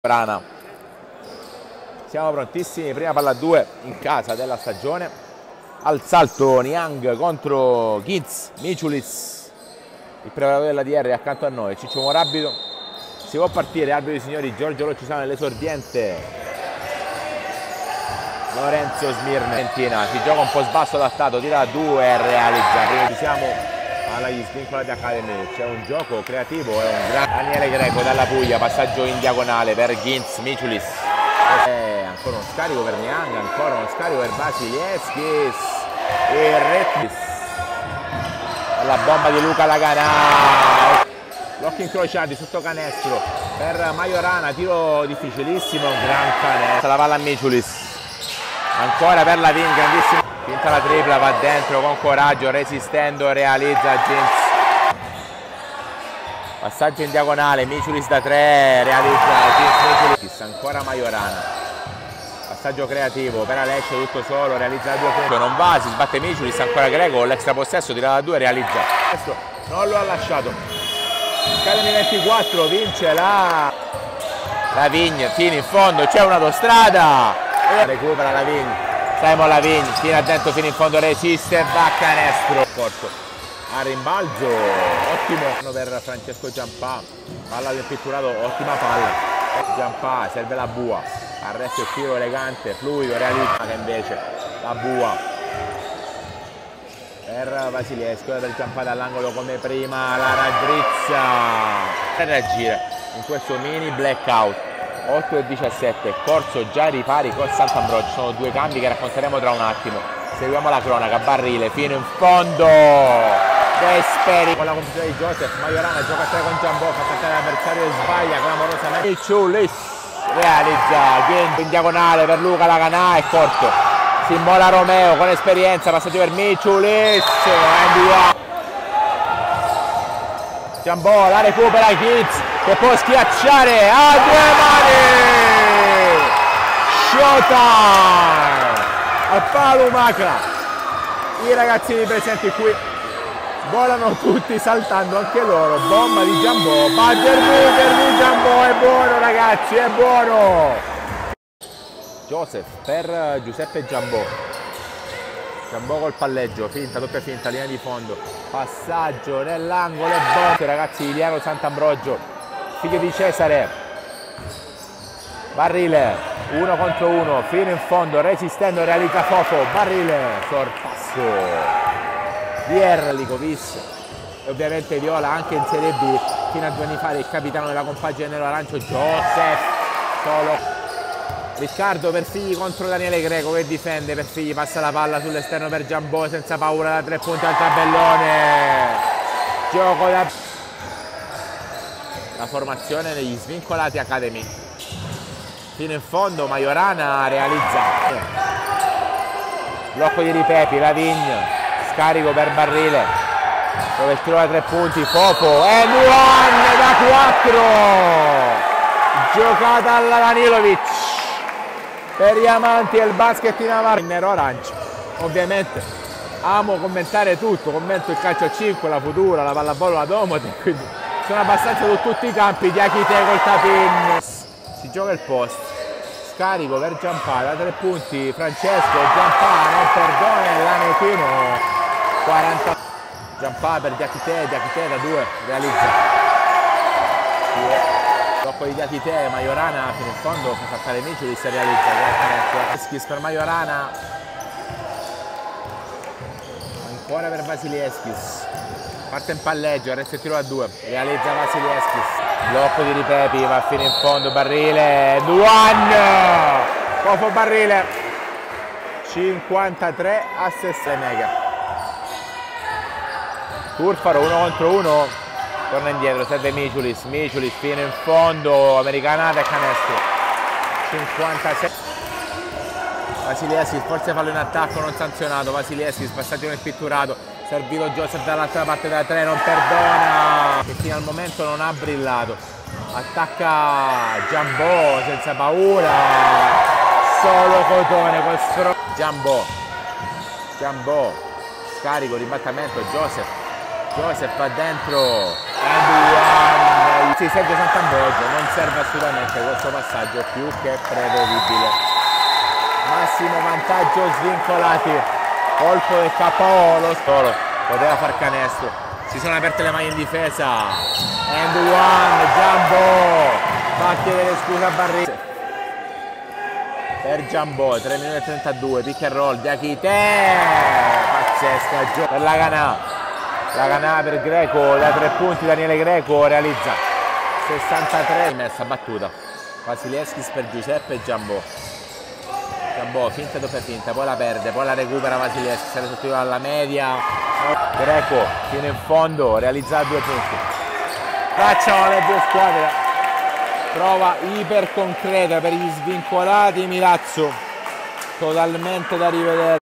DIERRE- SVINCOLATI ACADEMY
Le migliori azioni dalla sfida del PalaLumaka- Telecronaca di Giovanni Mafrici.Ph Fafo